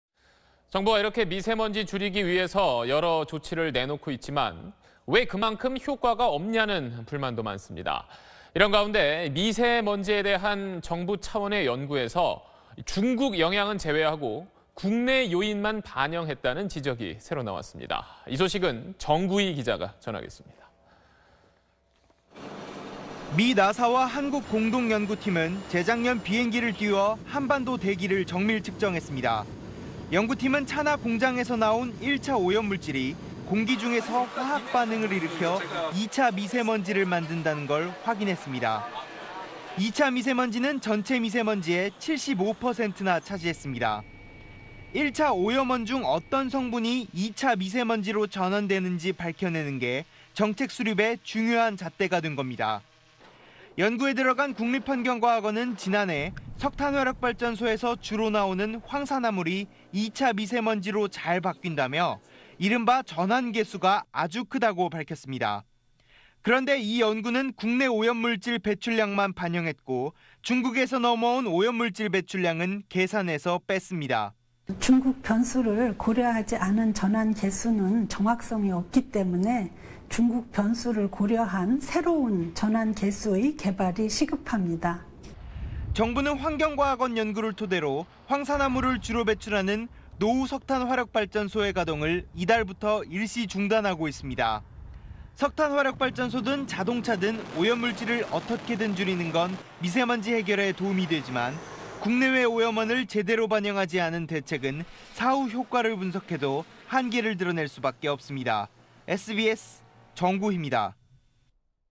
정부 미세먼지 연구, 중국 오염물질 영향은 뺐다 / SBS
이렇게 정부가 미세먼지 줄이기 위해 여러 조치를 내놓고 있지만 왜 그만큼 효과가 없냐는 불만도 많습니다. 이런 가운데 정부 차원의 연구에서 중국 영향은 제외하고 국내 요인만 반영했다는 지적이 새로 나왔습니다.